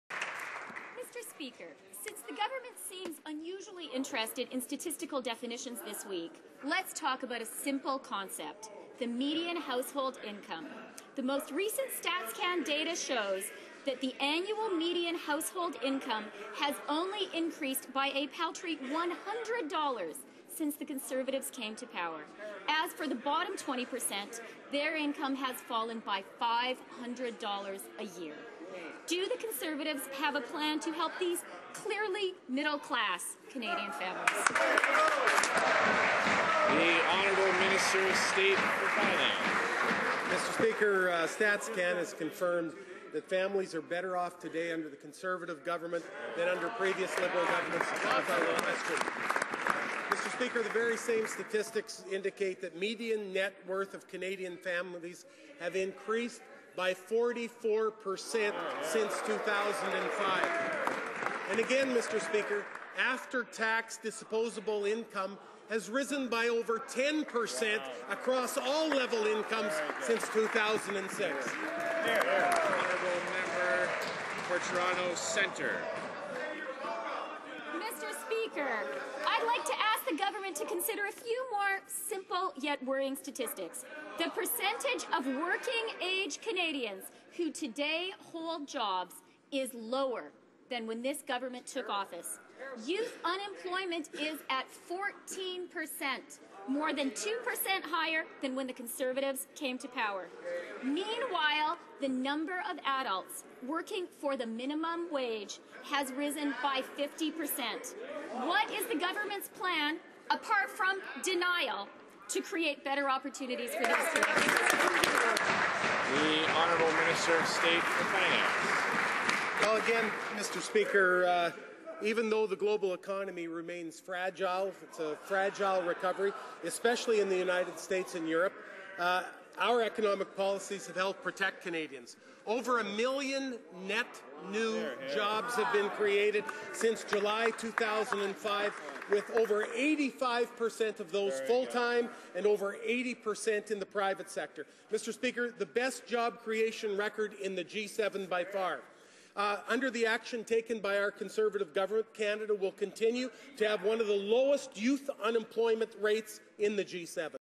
MP Chrystia Freeland on middle class income and youth unemployment
MP Chrystia Freeland challenges the government on its record on middle class income and youth unemployment in Question Period.